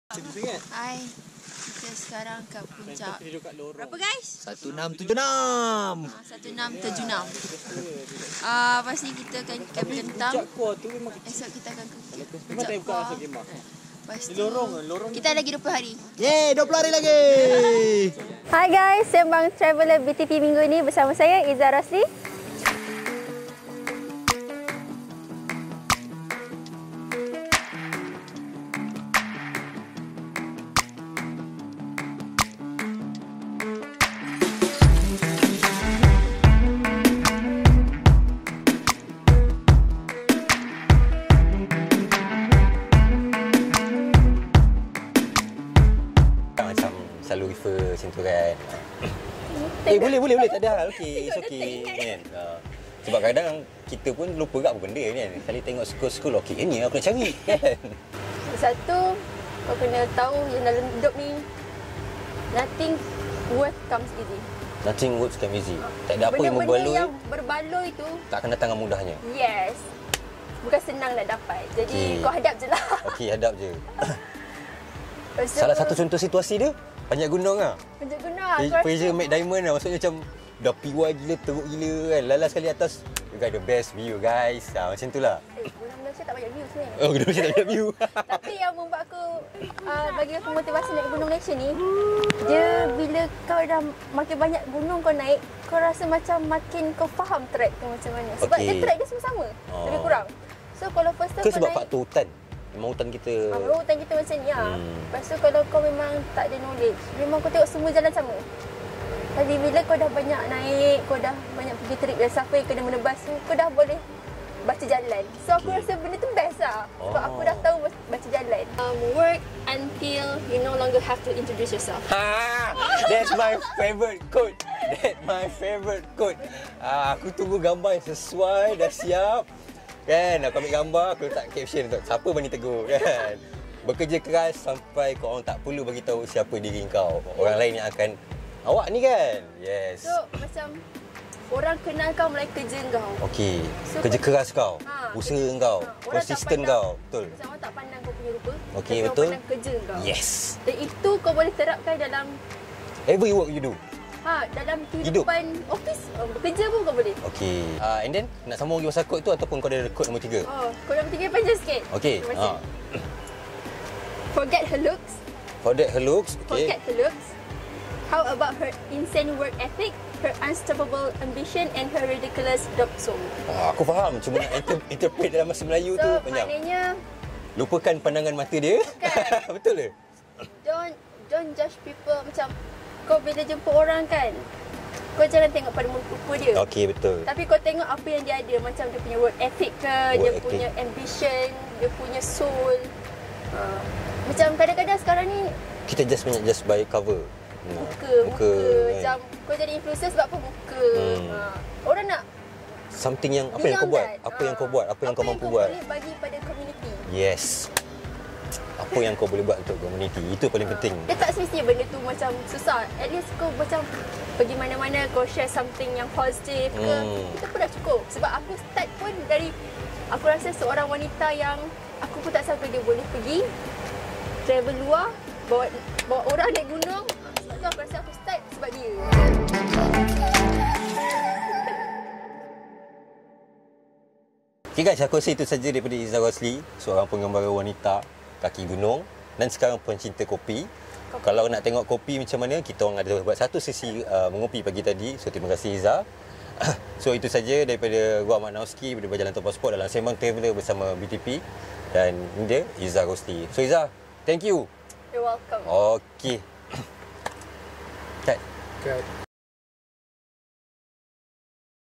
BTP - Sembang Traveller BTP Izza Rosli - Orang Kenal Kau Sebab Kerja Keras Kau // Part 6
Izza Rosli merupakan seorang wanita yang bergerak solo untuk mendaki gunung disekitar Malaysia termasuk Indonesia dan Jepun.

Pengarah
Matt Nauski

Videografer
Mah

Terima kasih kepada penaja .

Lokasi
Gibs Eco Tourism Resort

Pakaian
Istikoma